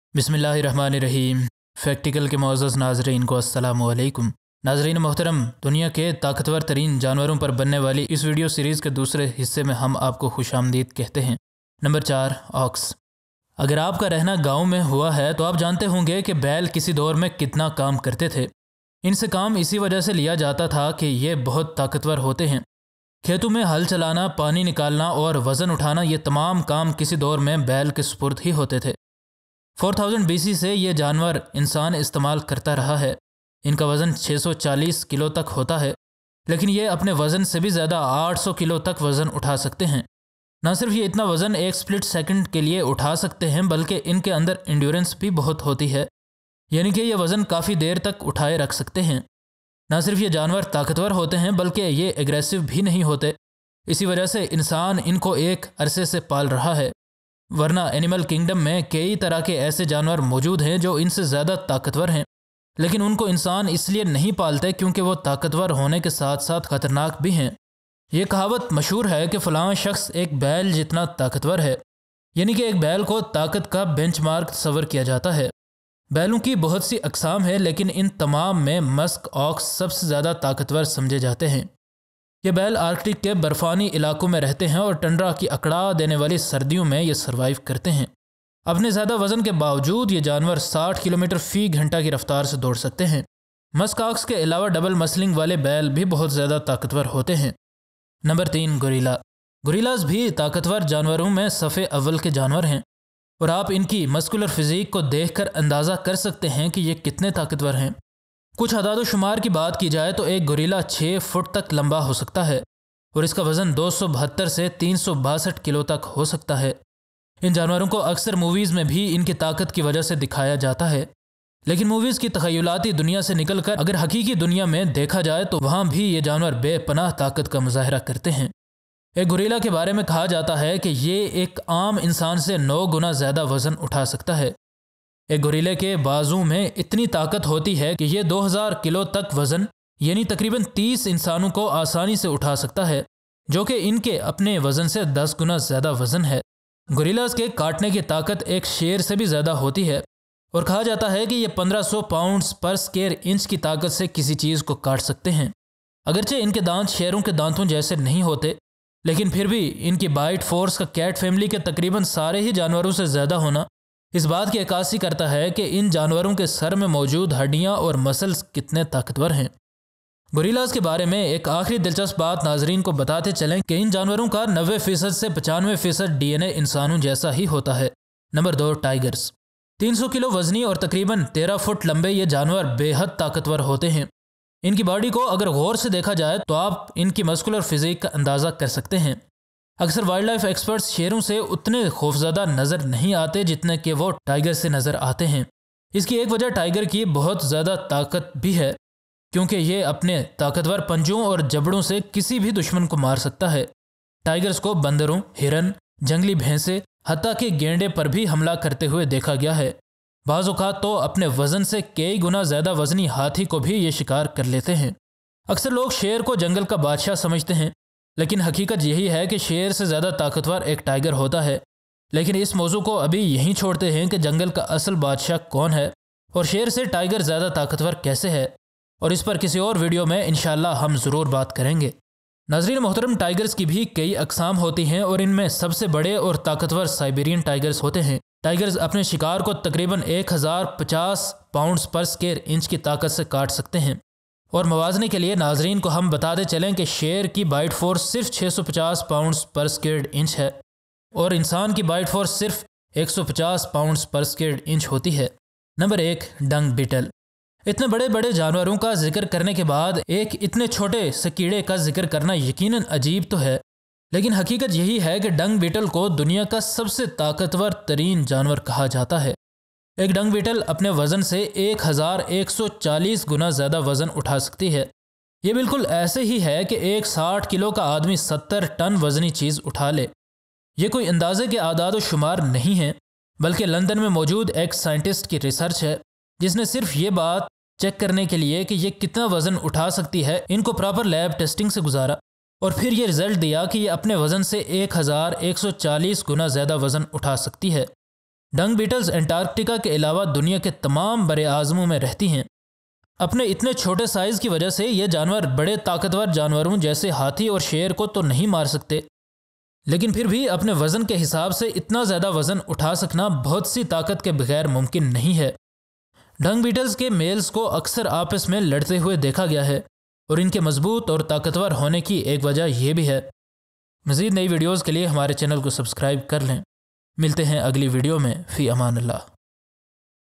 Most Powerful Animals in the World Part 2
Animals have brute force to do anything but not all of the animals are created equal some have power while some are weak. In Part 2 of the Most Powerful Animals in the World we will tell you about another set of 4 animals that are super strong and super powerful. The number 4 is ox which is used for pulling, driving the carts and plowing the fields. The ox has great power and can pull almost 80 kgs of weight. The most powerful animal among the ox is Musk Ox which lives in the Siberia. The number of 3rd most powerful animal is Gorilla. Gorillas have a muscular physique and have bite force more than that of most of the cat family. Number 2nd is Tiger which is also very powerful. The number is dung beetle which can lift 1140 times weight as compared to its body weight and hence is considered the most powerful animal in the world.

For Brand Promotion and other Queries